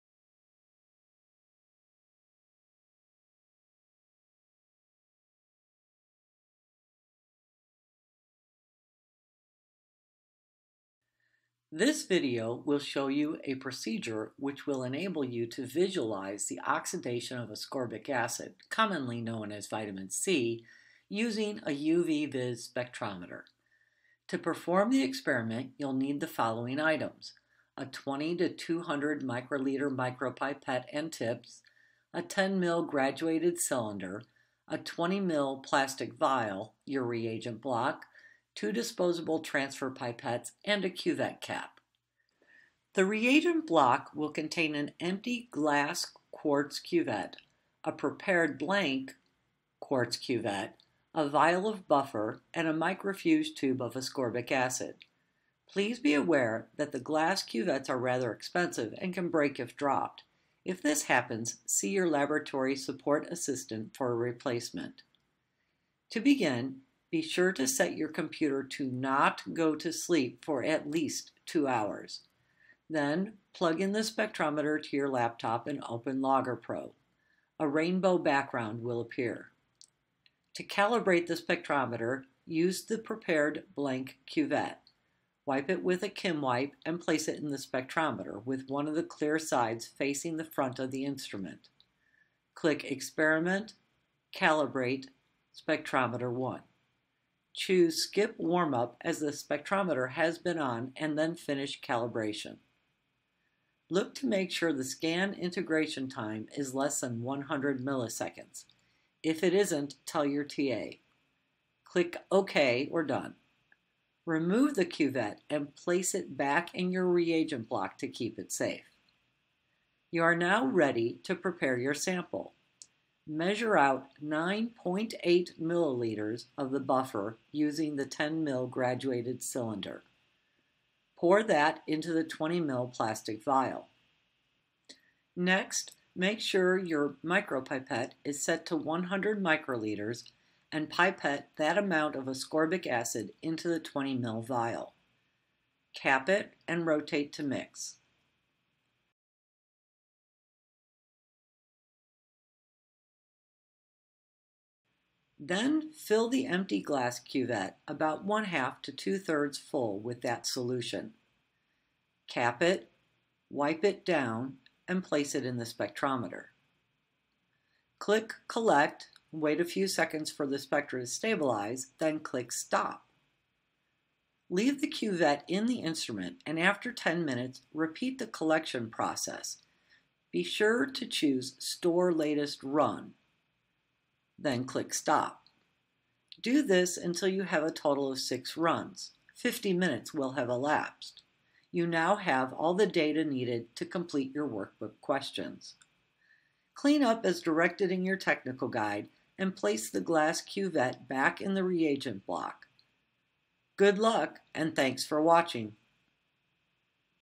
Oxidation of Vitamin C
This video shows the oxidation of vitamin C over time by UV spectrometry.